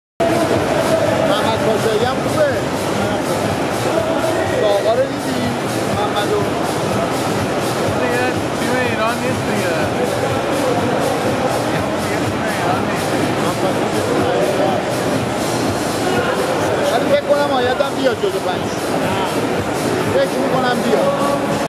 Ayat Nadjdbaghemmani, Chris Jones, Edwin Narnor, Sayed Hameed - Arnold Amateur Europe 2014
Ayat Nadjdbaghemmani – Competitor No 531 – Position 2nd
Chris Jones – Competitor No 532 – Position 13th
Edwin Narnor – Competitor No 534 – Position 10th
Sayed Hameed – Competitor No 535 – Position 4th
Prejudging
Posing
IFBB
Under 90kg
Arnold Europe Amateur 2014
Bodybuilders
Bodybuilding